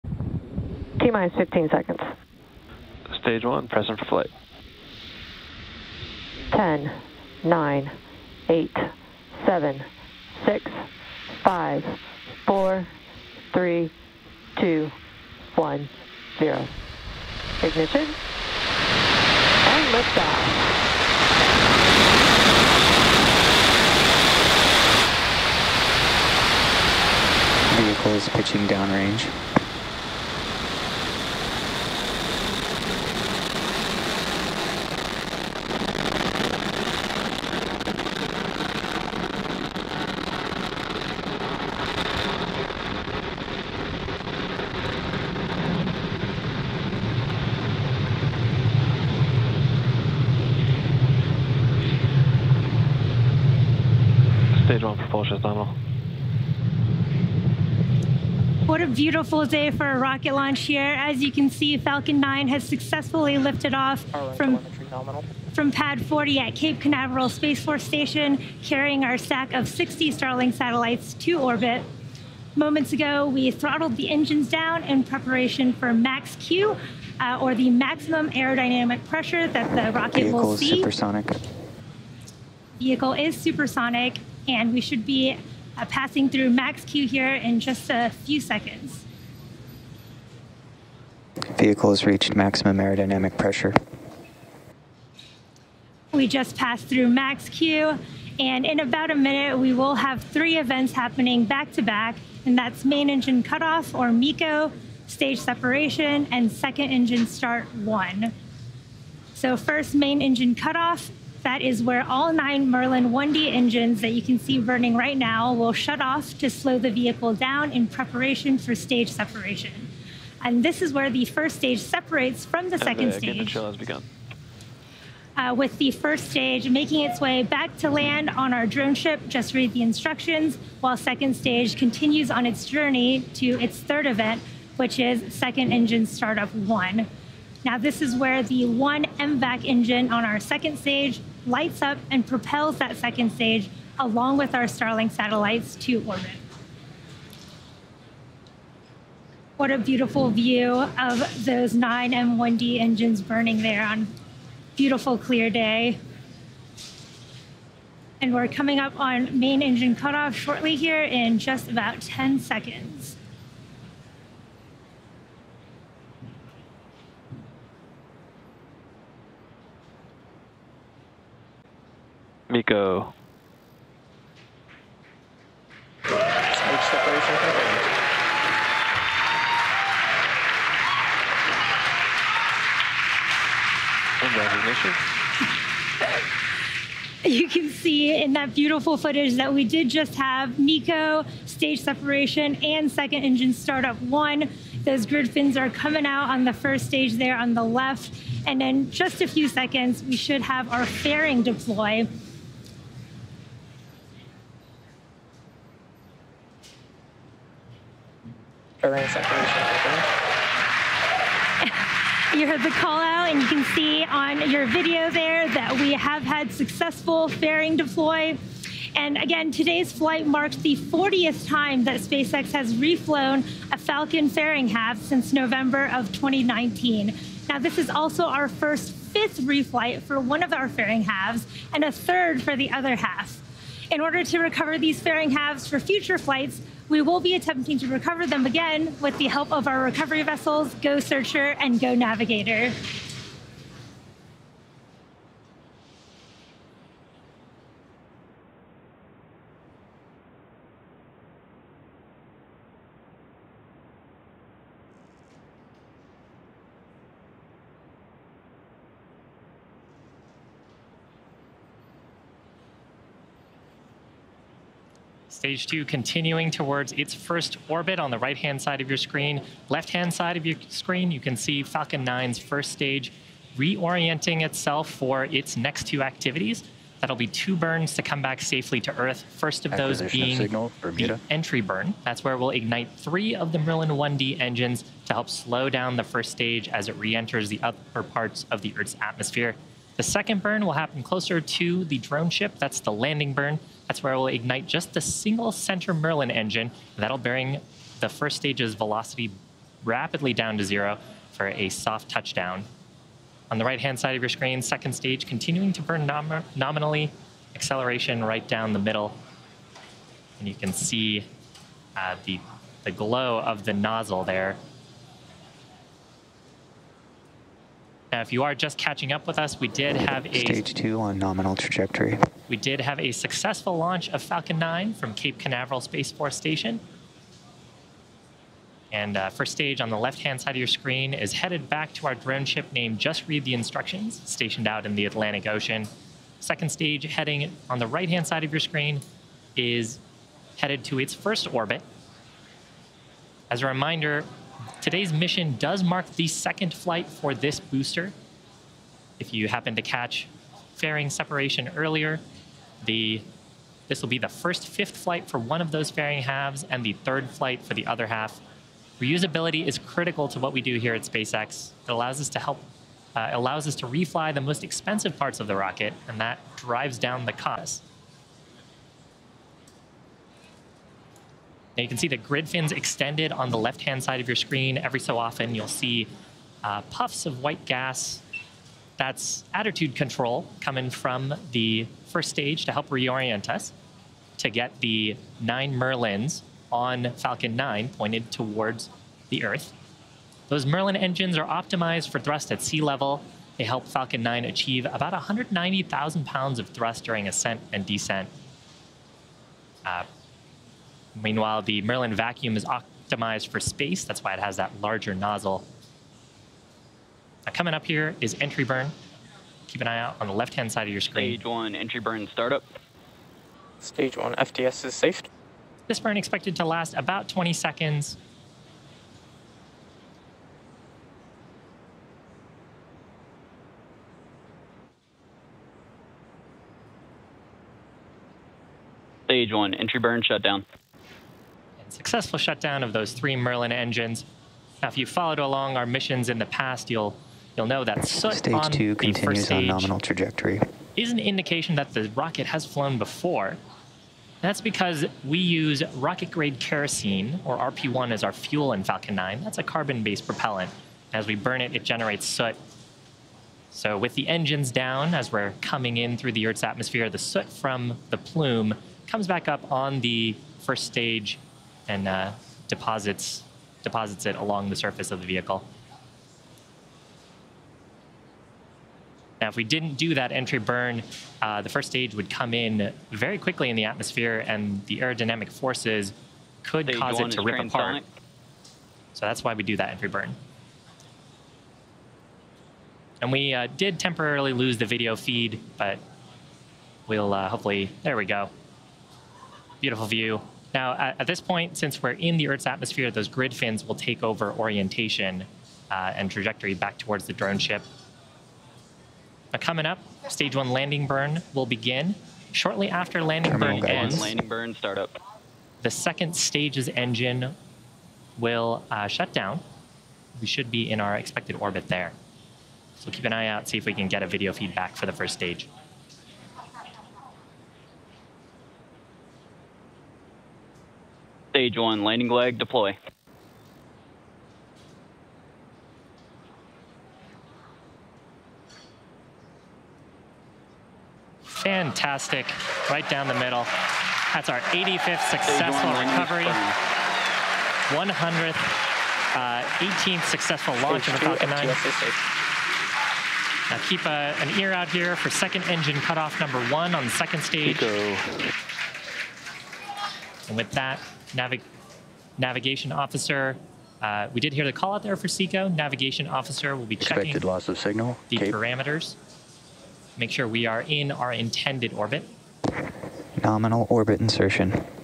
SpaceX Starlink-28 launches! (carrying 60 Starlink satellites on-board)
SpaceX Falcon 9 sends to orbit 60 Starlink satellites from SLC-40 in Cape Canaveral and then nails the landing. Watch it here.